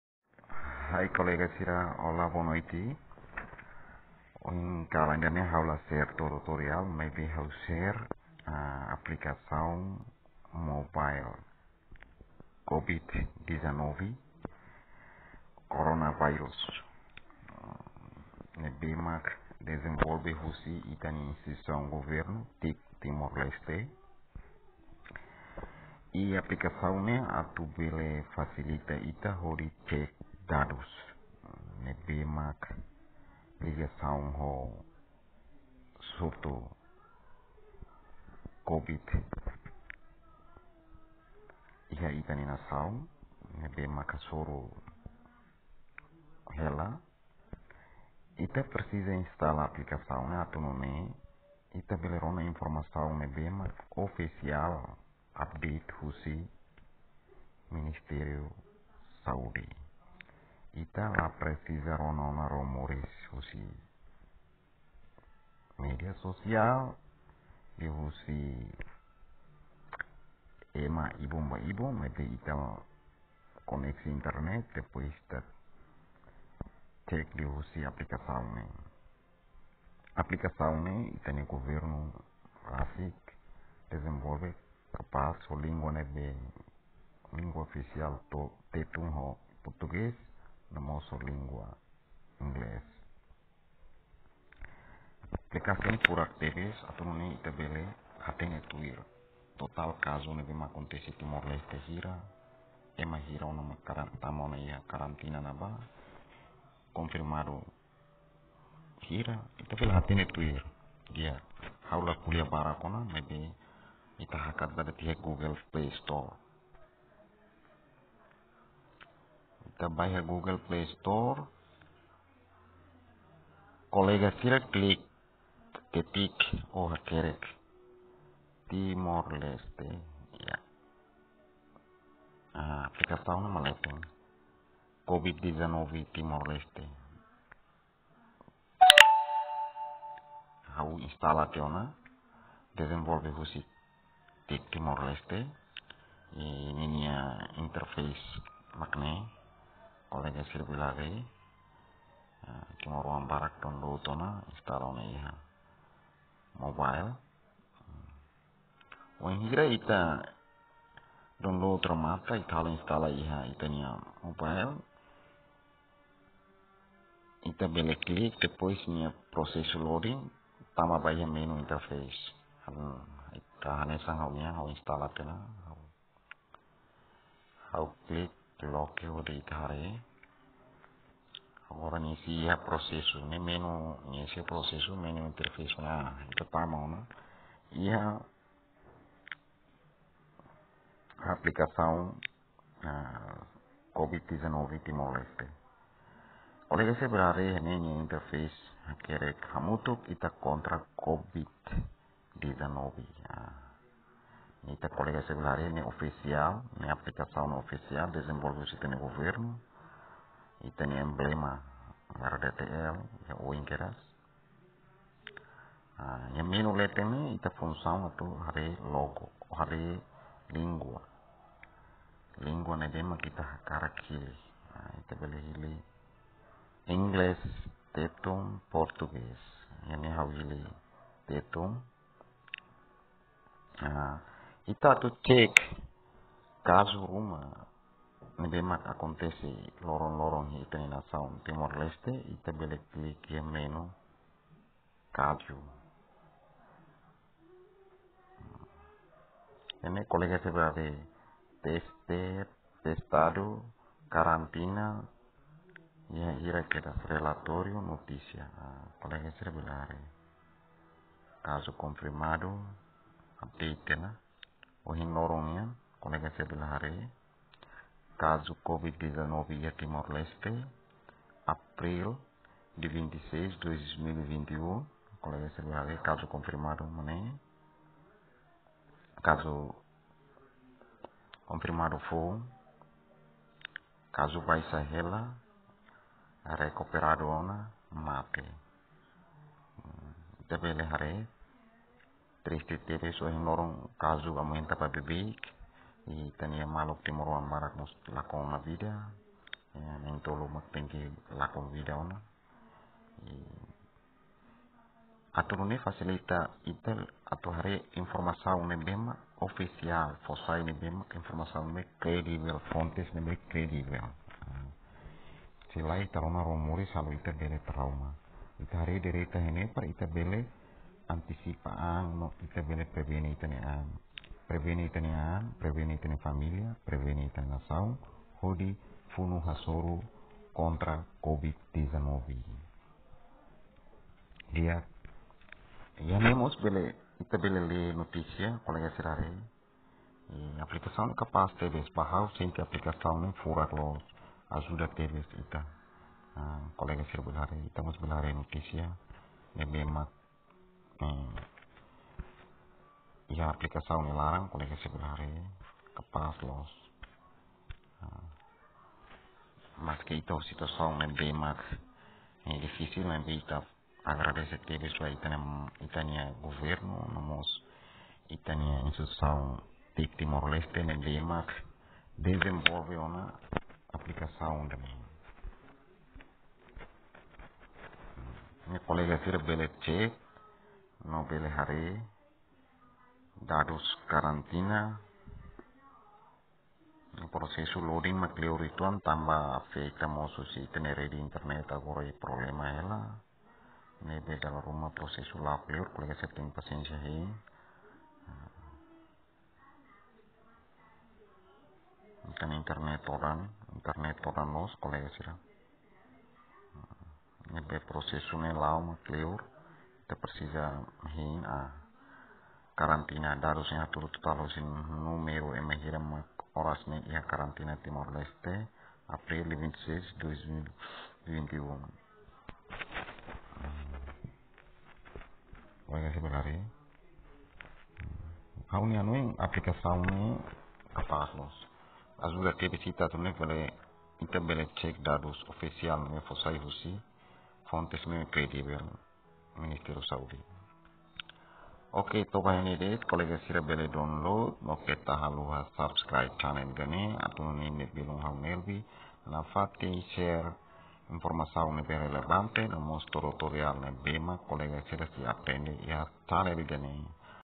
Aplikasaun Mobile Update Dadus Covid-19 iha TLS🇹🇱
Aplikasaun Mobile Update Dadus Covid19 iha TL, objetivu atu fasilita no ajuda Utilizador bele asesu informasaun ne'ebe mak fontes kridevel husi  Ministeriu Saude🏥🇹🇱🏥